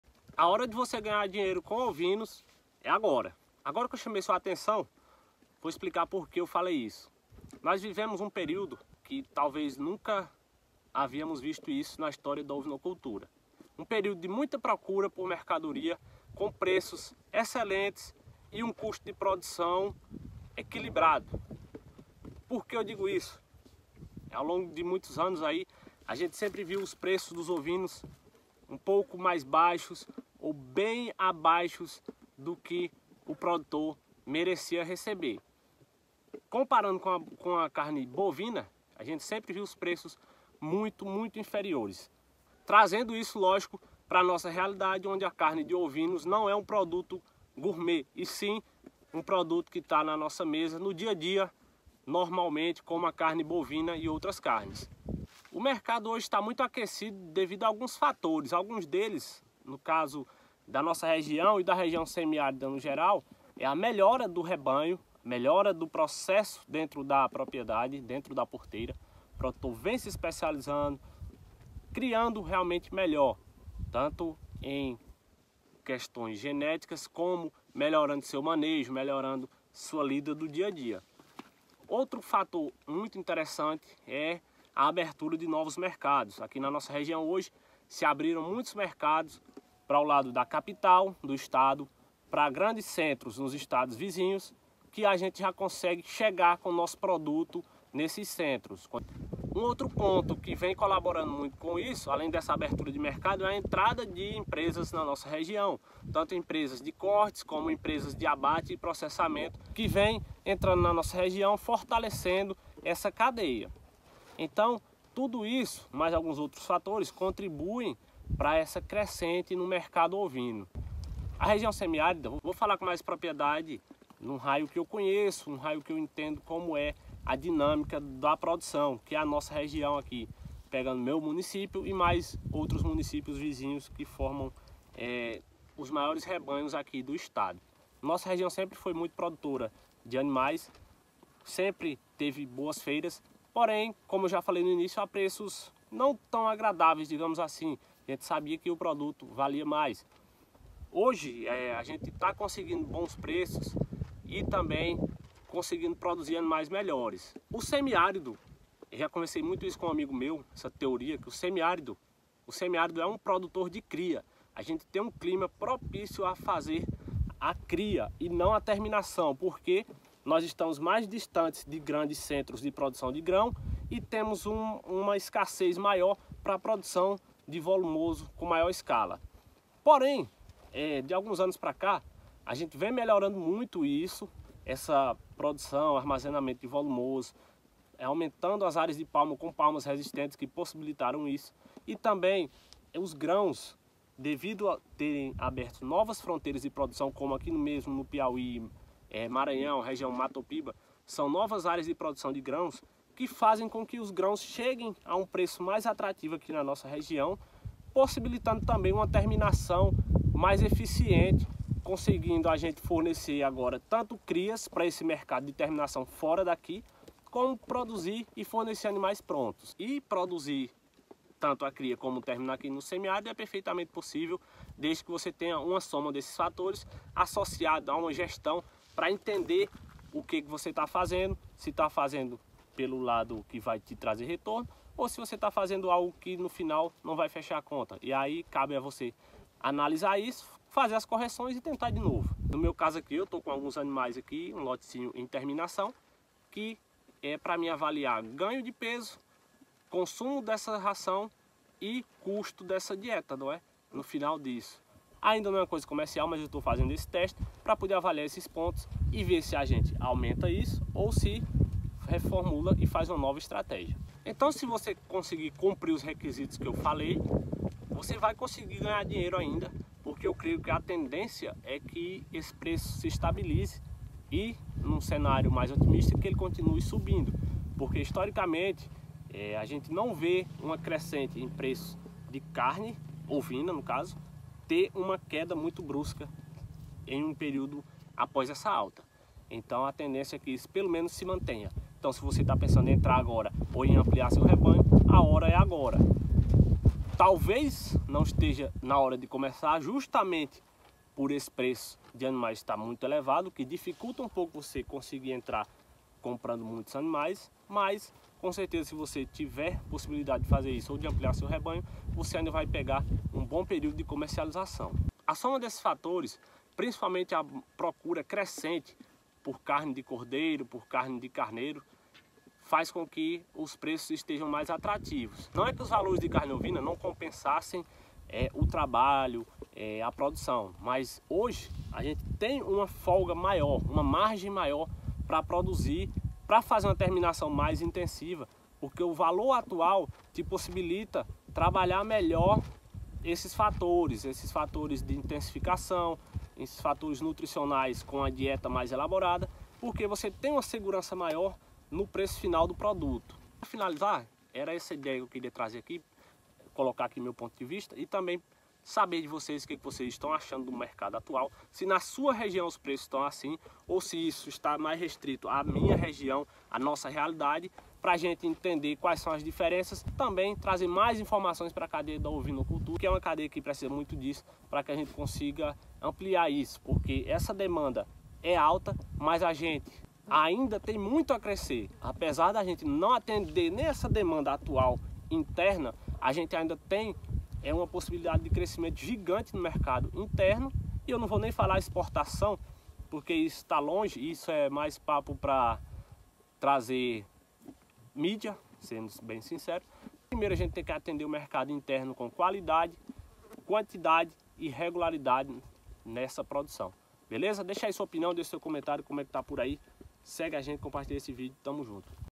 MERCADO ATUAL DA OVINOCULTURA
Neste vídeo falo um pouco de como está o mercado dos ovinos na nossa região.

Os valores atuais dos animais e da carne de ovinos atinge marcos interessantes que fazem com que produzir ovinos (ovelhas e carneiros) seja uma excelente opção para ganhar dinheiro.